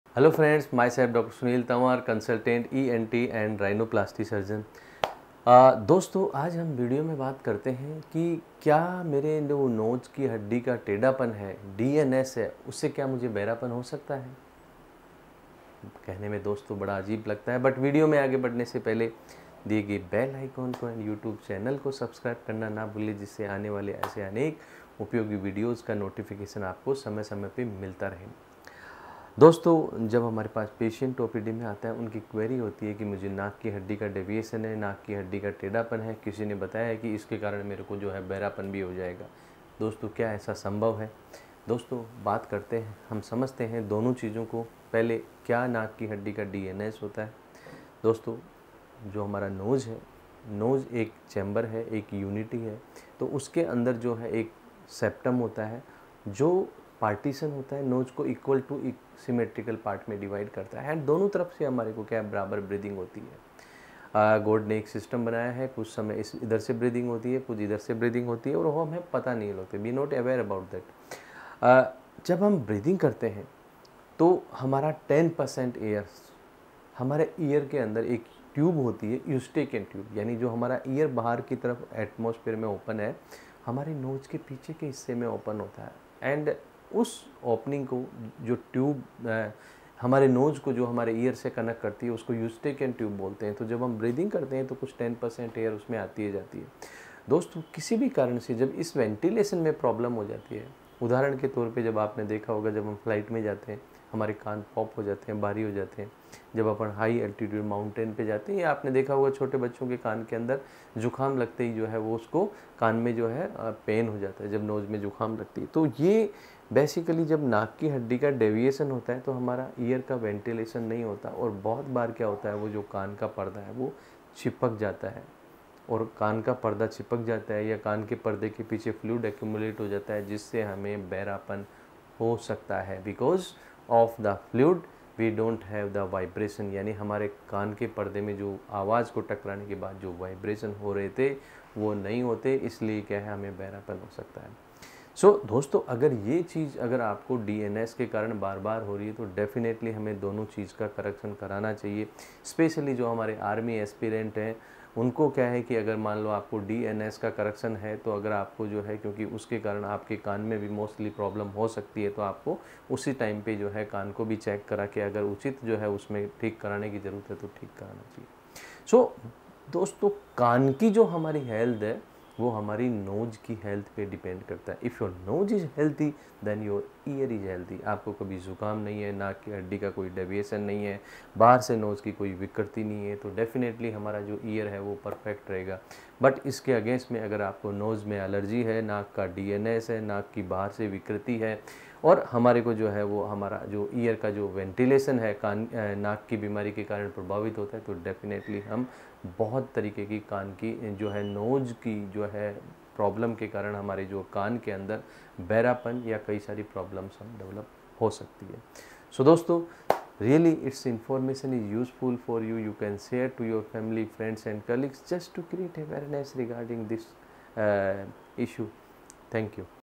क्या nose Ke Tedepan Se Behrepan Bhi Ho Sakta Hai? | Can deviatednasalseptum Cause Deafness?
क्या नाक के टेडेपन से आपका बेहरापन बढ़ सकता है? इस हिंदी वीडियो में, हम आपको नाक के टेडेपन और बेहरापन के बीच के संबंध के बारे में सभी महत्वपूर्ण जानकारी प्रदान करेंगे। जानें कैसे नाक के टेडेपन का इलाज और सावधानियाँ बेहरापन से निपटने में मदद कर सकती हैं।

आपके सवालों के लिए हमारे विशेषज्ञ डॉक्टर की सलाह और सुझाव सुनें और जानें कि क्या नाक के टेडेपन से जुड़े असली तथ्य क्या हैं।

इस वीडियो को देखें और नाक स्वास्थ्य के मामले में अपनी जानकारी को बढ़ावा दें।